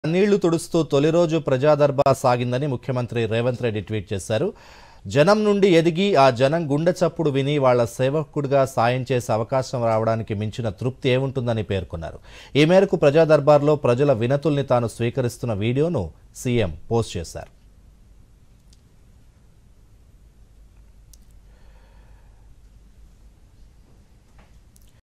జనం కష్టాలు వింటూ, కన్నీళ్లు తుడుస్తూ తొలి ప్రజా దర్బార్ జరిగిందని ముఖ్యమంత్రి రేవంత్ రెడ్డి ట్వీట్
జనం కష్టాలు వింటూ, కన్నీళ్లు తుడుస్తూ తొలి  ప్రజా దర్బార్సాగిందని ముఖ్యమంత్రి రేవంత్ రెడ్డి  ట్వీట్ చేశారు.  ఆఇనం నుండి ఎదిగి,  ఆ జనం గుండె చప్పుడు విని, వాళ్ల సేవకుడిగా   సాయం చేసే అవకాశం  రావడానికి  మించి తృప్తి ఏముంటుందని పేర్కొన్నారు.  ఈ మేరకు   ప్రజా దర్బార్లో ప్రజల వినతులను తాను స్వీకరిస్తున్న వీదియోను పోస్టు చేశారు